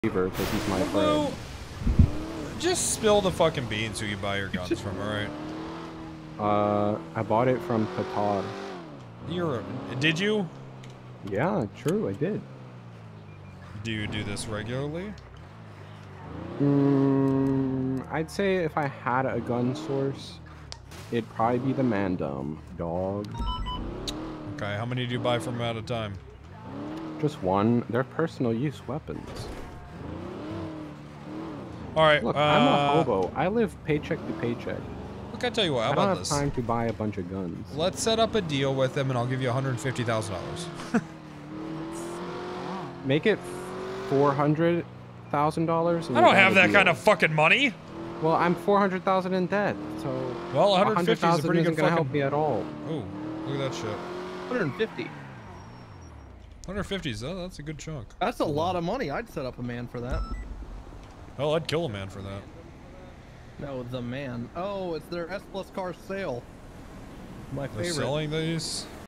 Slick Snitches on The Mandem to Wrangler
Slick snitches on Patar and Mandem when asked where he got his deagle.